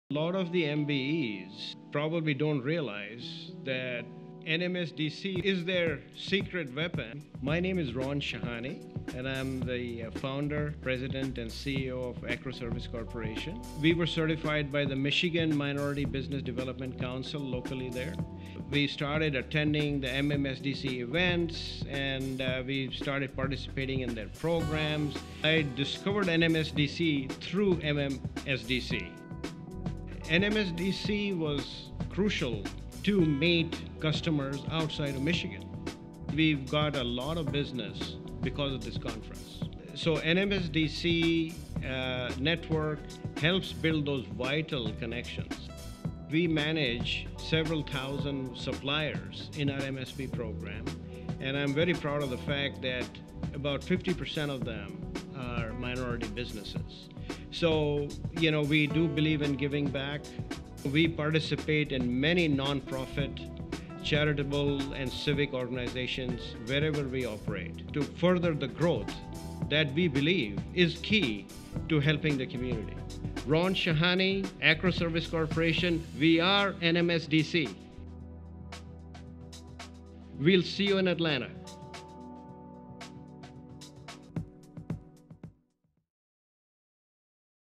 NMSDC Conference 2018 Interviews - Ron Shahani, Acro Service Corporation
Ron Shahani
Founder, President, & CEO
Acro Service Corporation

Get insights into the NMSDC Network, advise on how to grow your reach, and the advantages of being an NMSDC-Certified business.